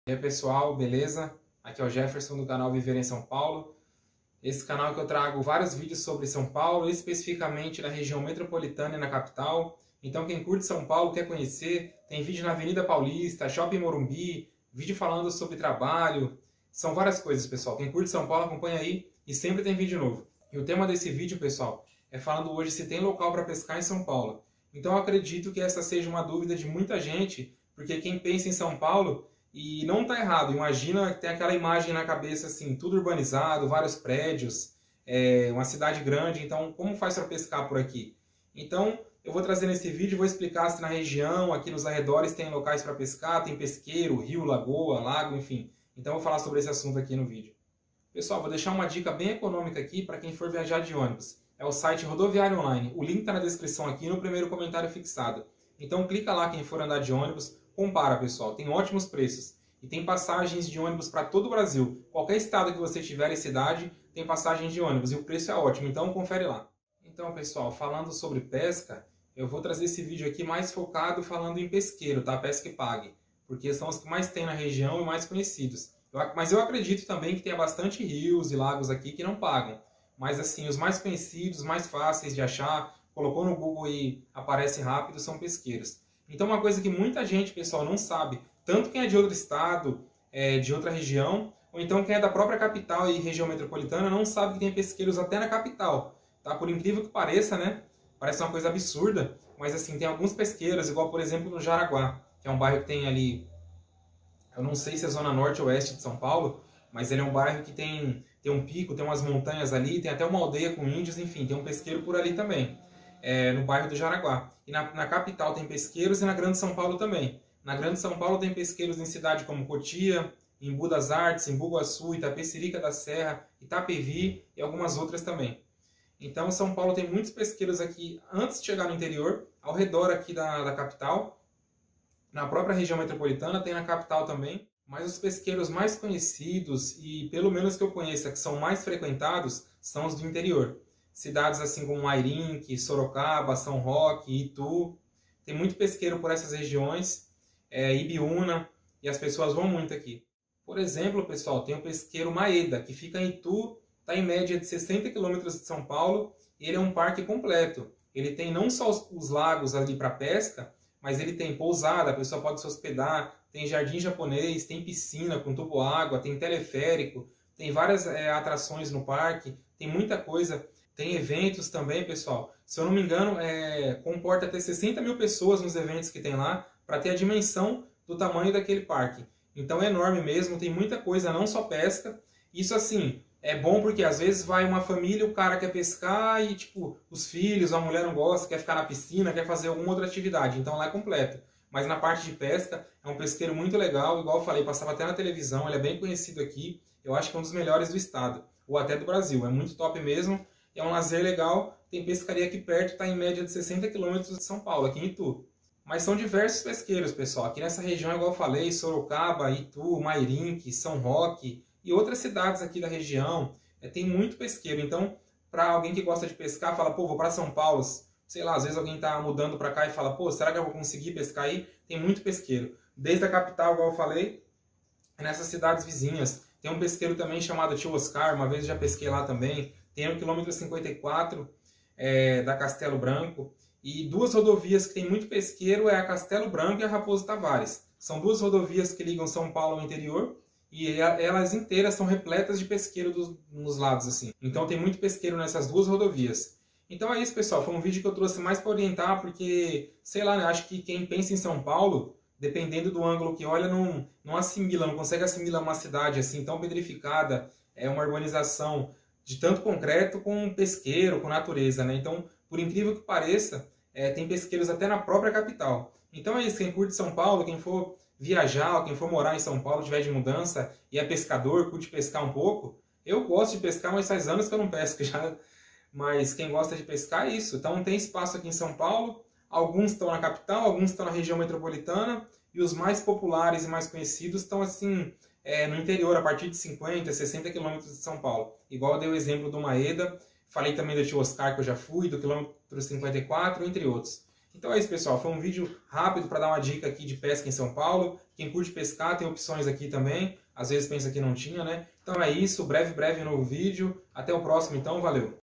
SÃO PAULO TEM LUGAR PARA PESCAR?
Mostrando sobre lugares para pesca em São Paulo, inclusive na própria capital...